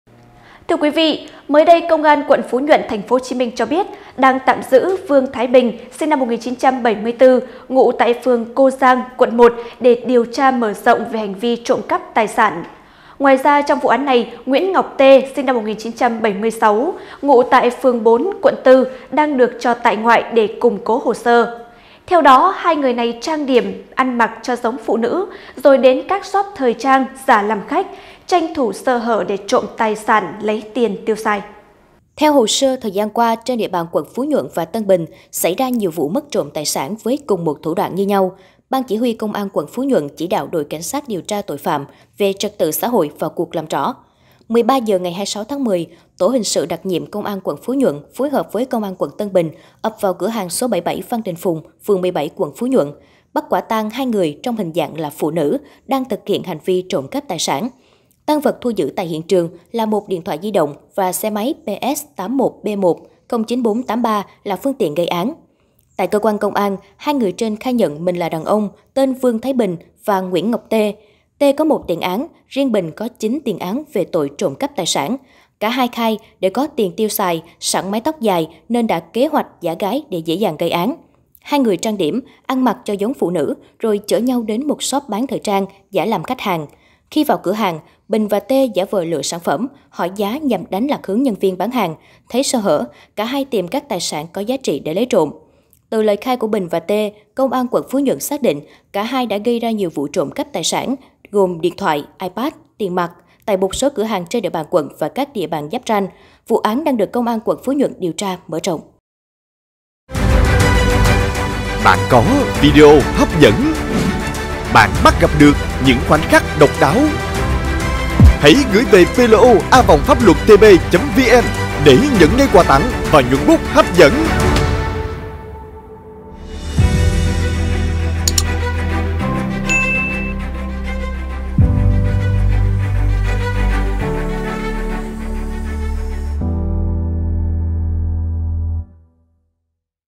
Bắt người có 9 tiền án giả dạng phụ nữ trộm tài sản tại nhiều cửa hàng ở TP.HCM - PLO
Hai người trang điểm, ăn mặc cho giống phụ nữ rồi đến các shop thời trang giả làm khách, tranh thủ sơ hở trộm tài sản để lấy tiền tiêu xài.